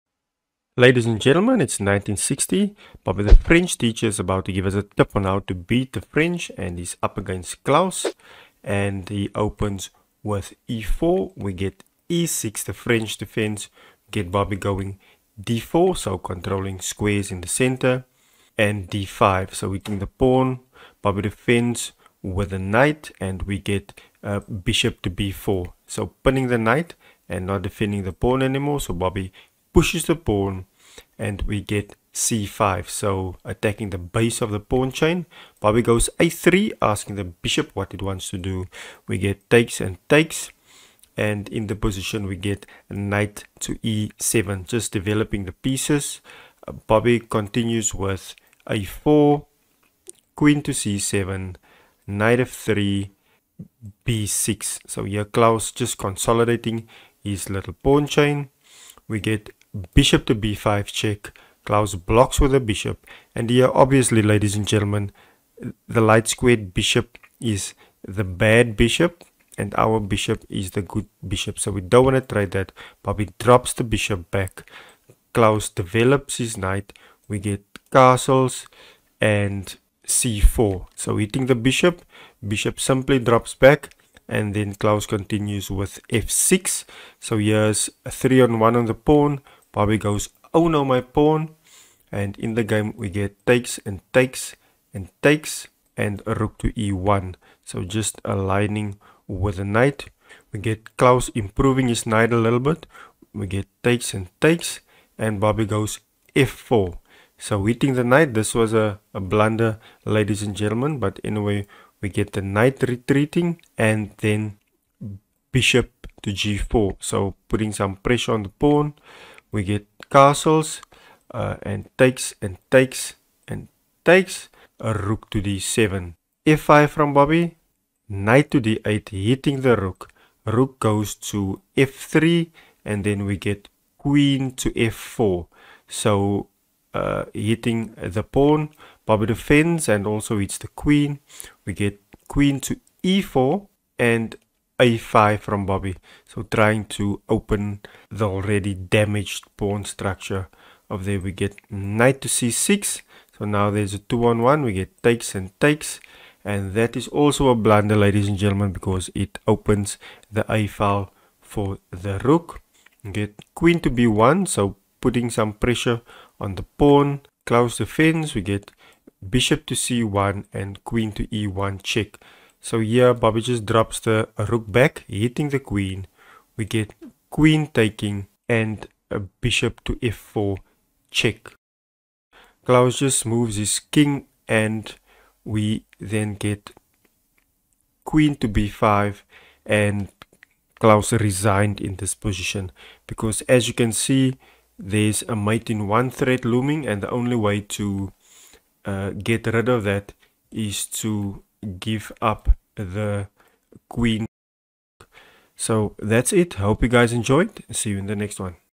Bobby Fischer - French Defense is just BAD
Bobby shows us how to play against the French defense